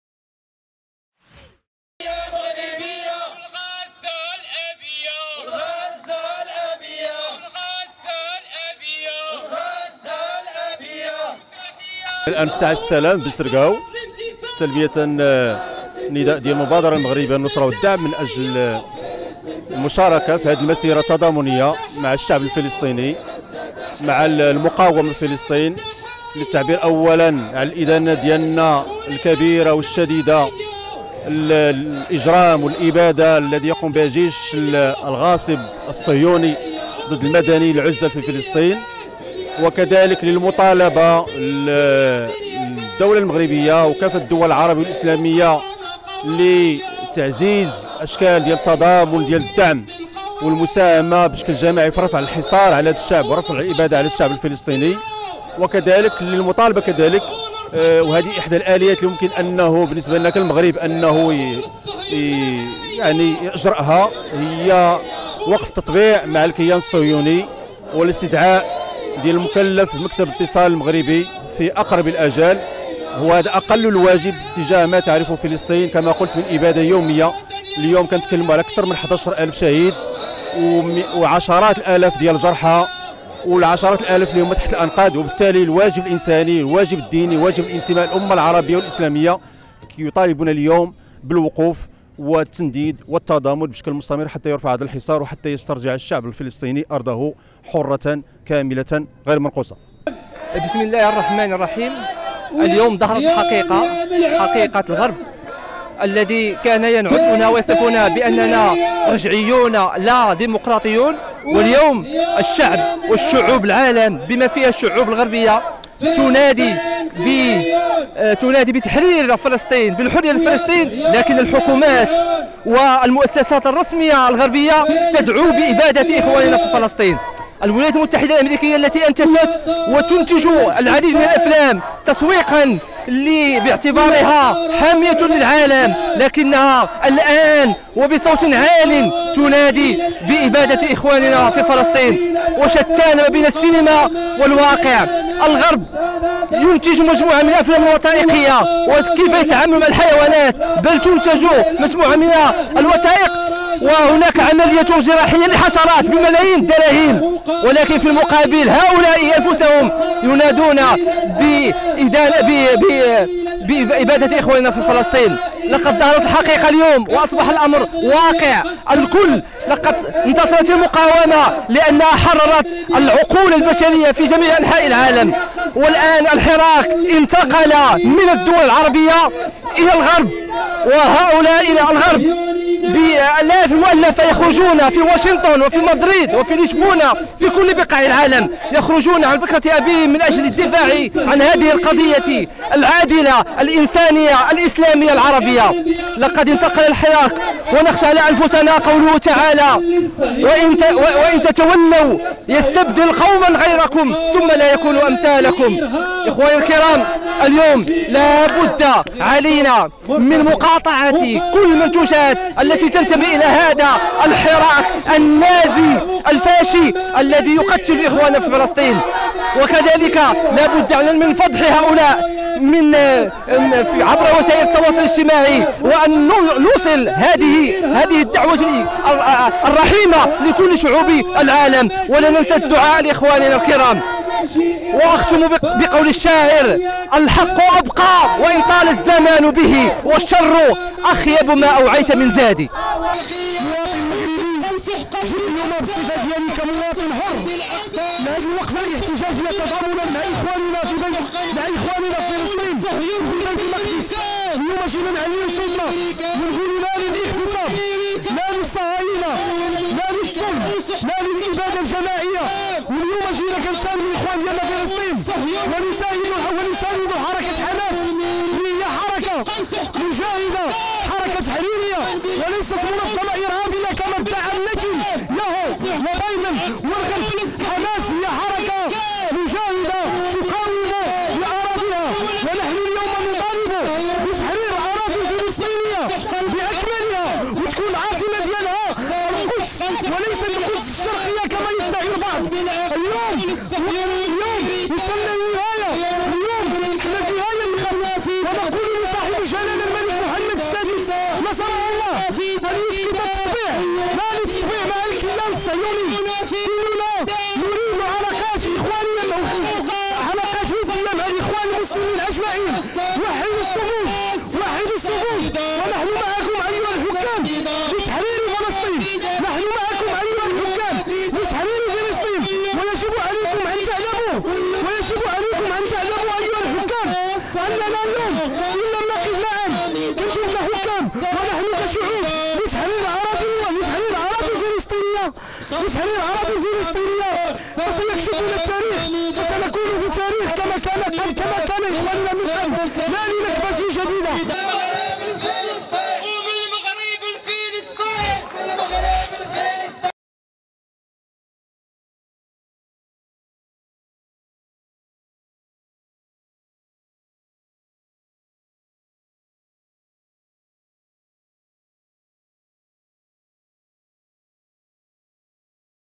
فعاليات بأكادير تندد باستمرار قصف "إسرائيل" للمدنيين في *غـــ.ـــزة*
فعاليات  بأكادير تندد باستمرار قصف إسرائيل للمدنيين في غـزة